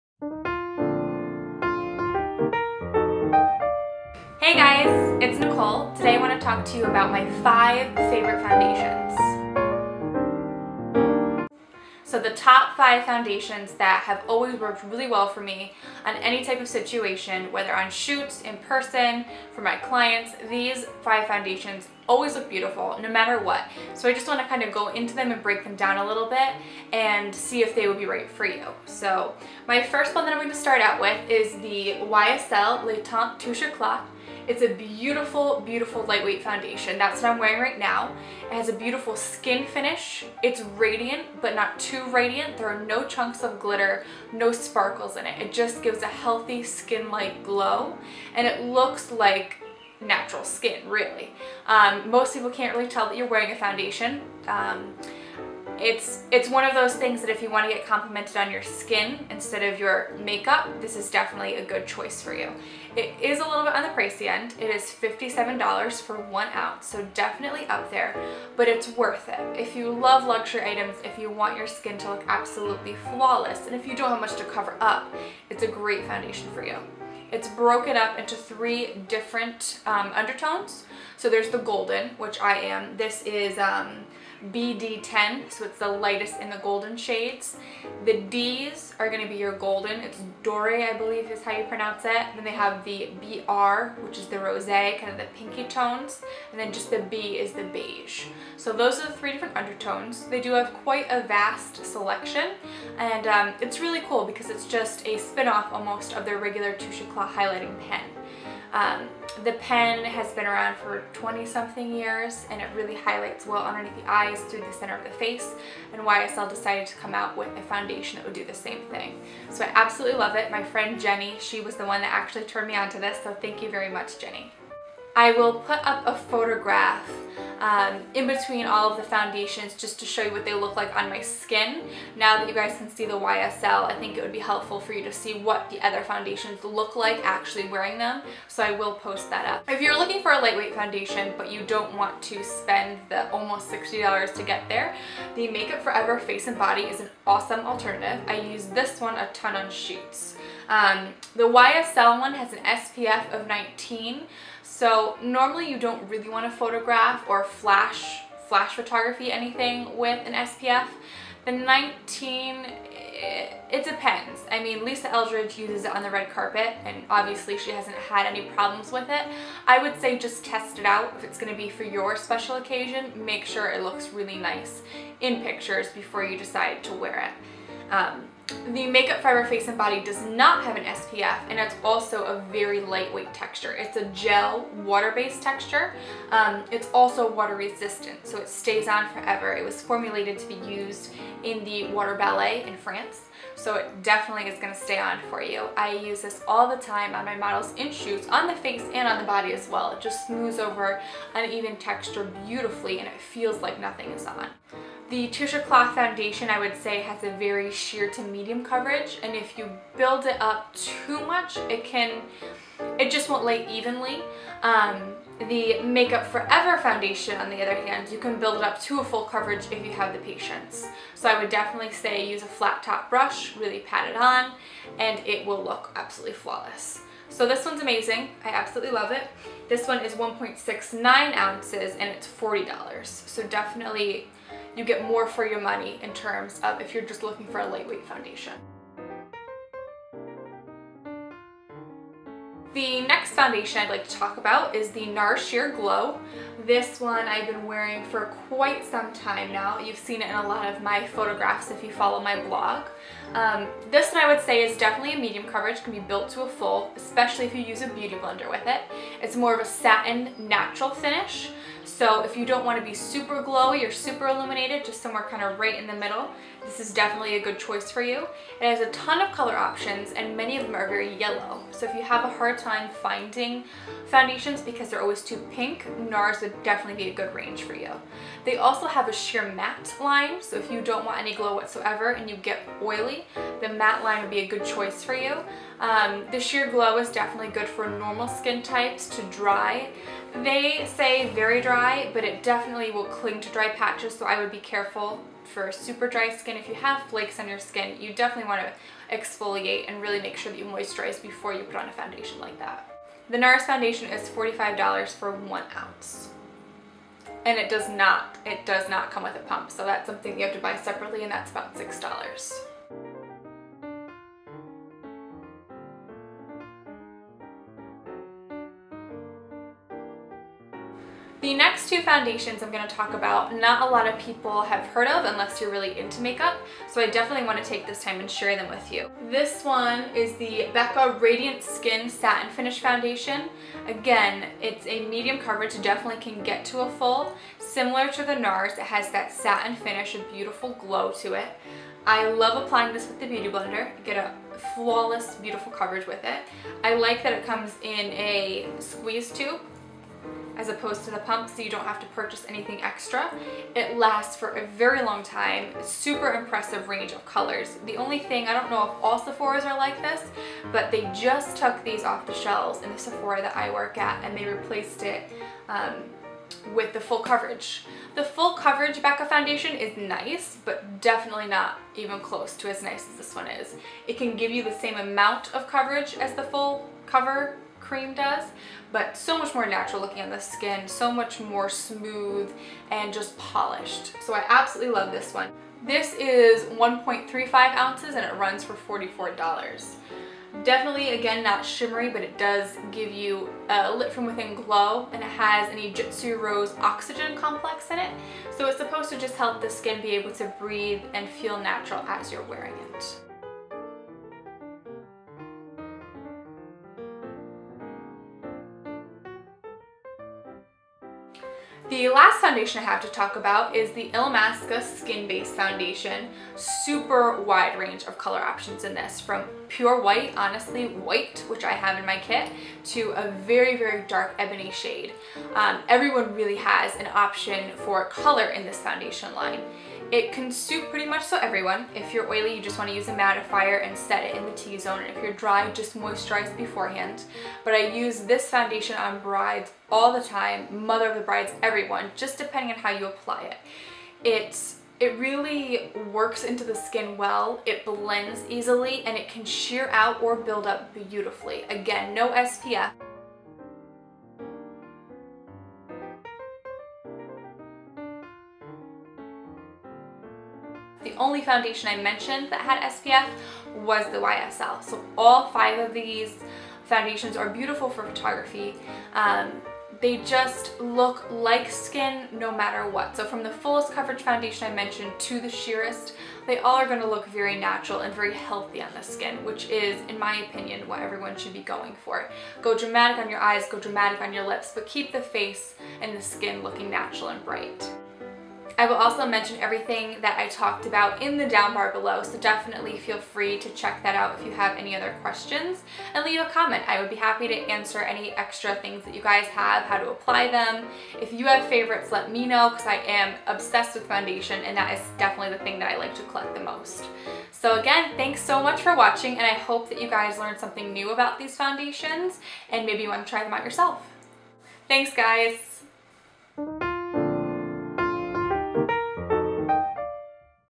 My Top 5 Favorite Foundations (great for photos)
A run down of my five favorite foundations that are also great in photography. List of foundations is below.

1. YSL: Le Teint Touche Eclat
2. Make Up For Ever: Face & Body
3. Nars: Sheer Glow
4. Becca: Radiant Skin Satin Finish
5. Illamasqua: Skin Base

FACEBOOK: Nicole Marie Makeup Artistry